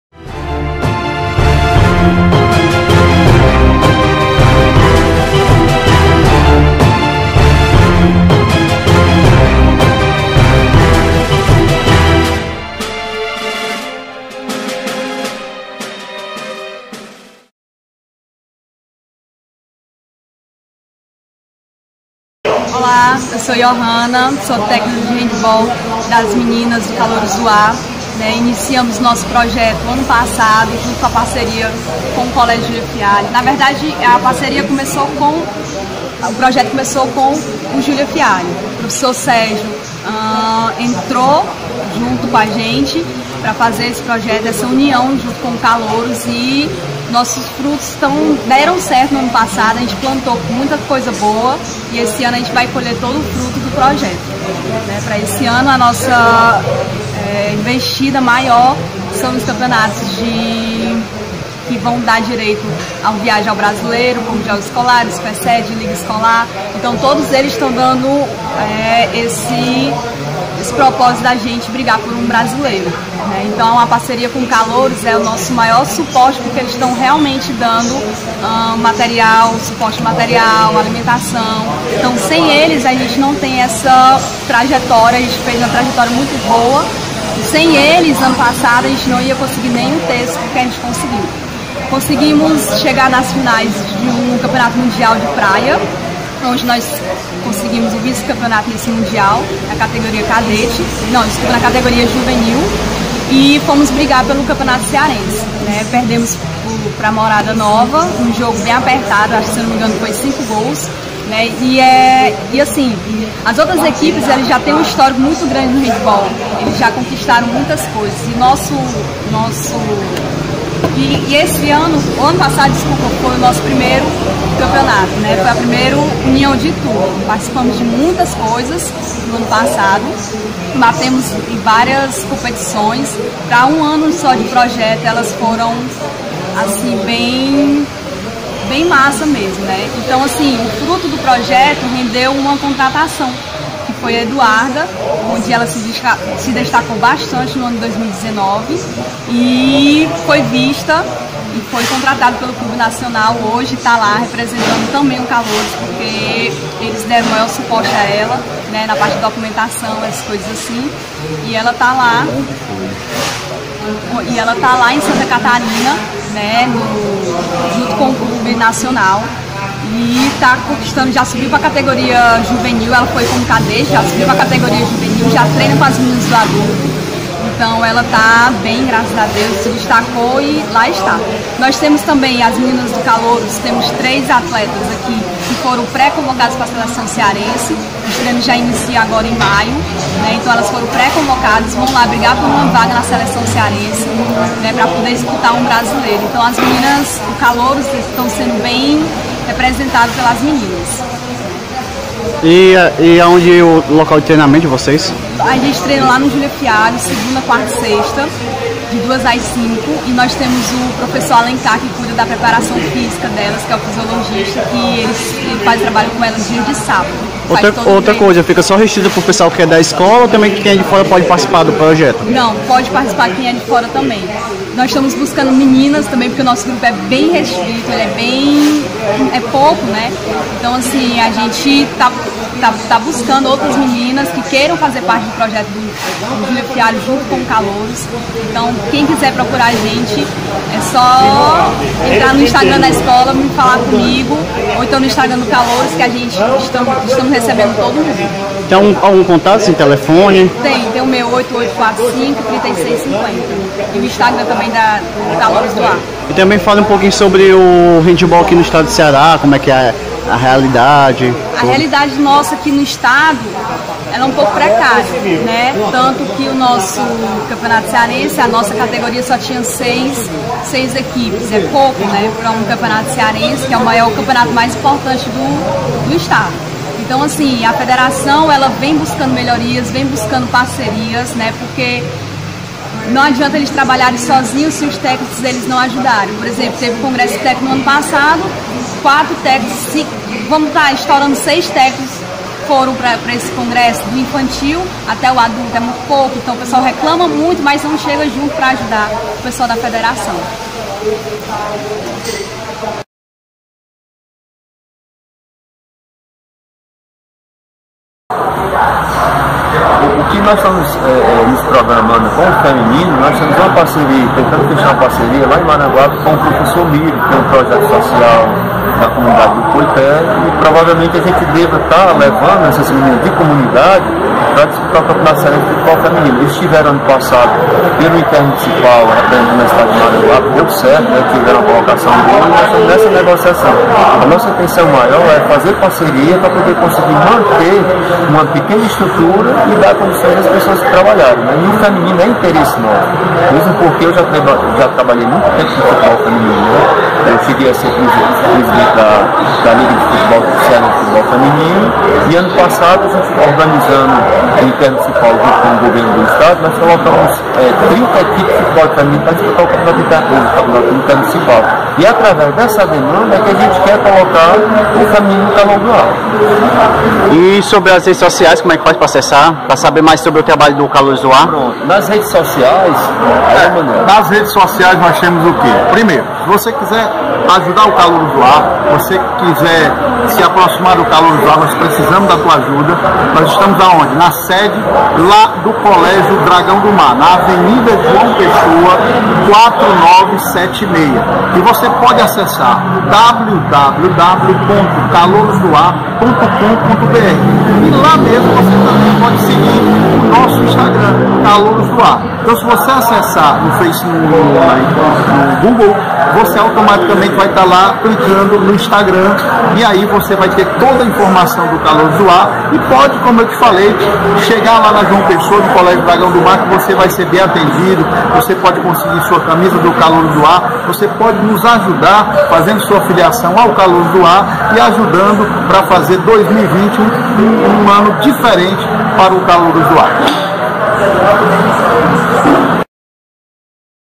[ Calouros do AR] Projeto para 2020 | BLOG Déber Lima e Portal Show de Esportes
Entrevista realizada no Bairro Dias Macedo, em Fortaleza.